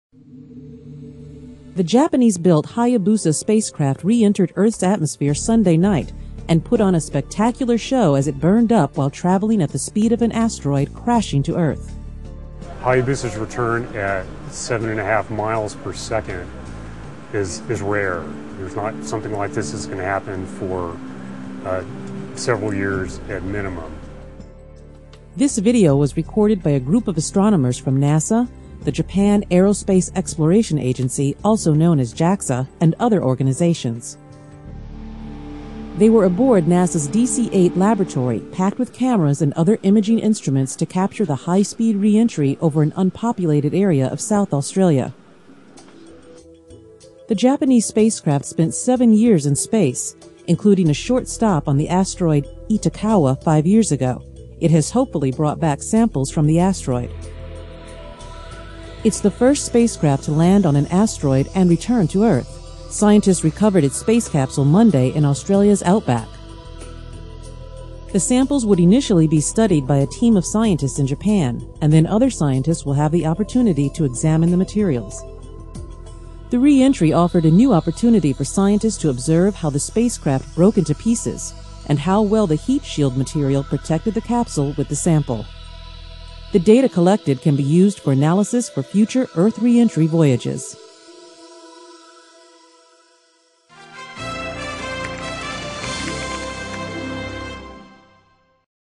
Video Hayabusa Spacecraft's Fiery Return From Asteroid
Japan's Hayabusa, the first spacecraft to land on an asteroid and return to Earth, made a fiery reentry over southern Australia Sunday night. Scientists hope the craft's capsule contains samples from the asteroid Itokowa.